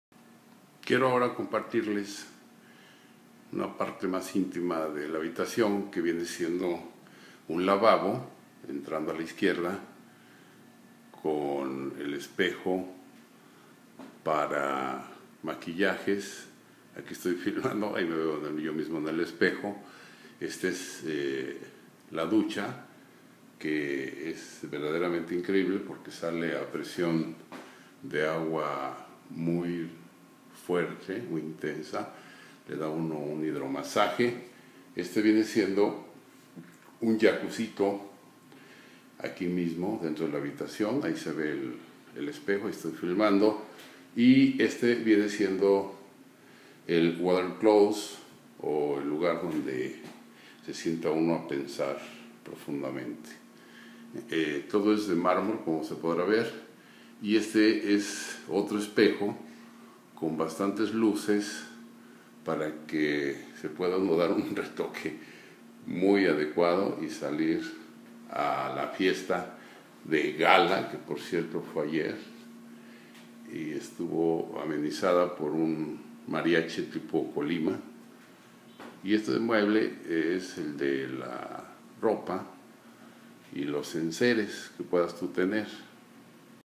BañoHabitaciónKarminaPalaceManzanilloMexico
Vista de 360o. del Baño de la Habitación 301 del Hotel Karmina Palace Manzanillo México, donde se celebró el 4o. Congreso de SuperCómputo del 5 al 8 de Marzo del 2013